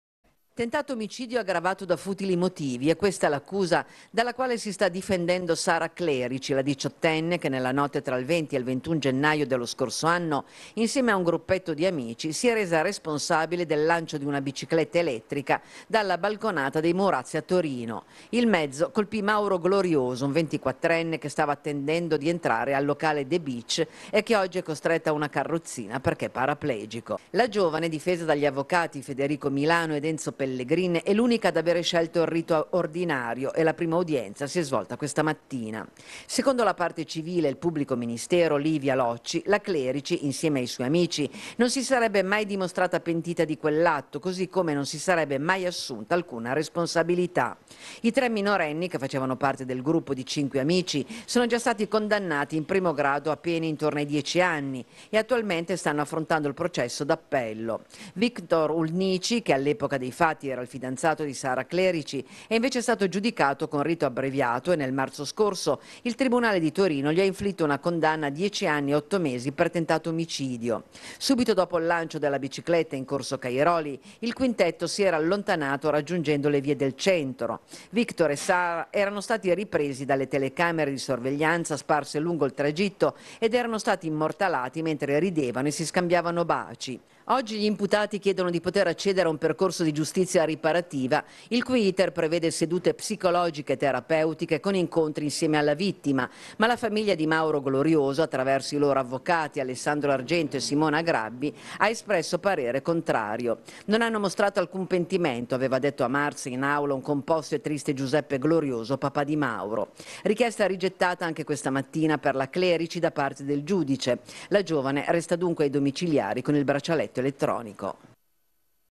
13/05/24 - Bici lanciata dai Murazzi: iniziato il processo per la 18enne Sara Clerici
13/05/24 - Bici lanciata dai Murazzi: iniziato il processo per la 18enne Sara Clerici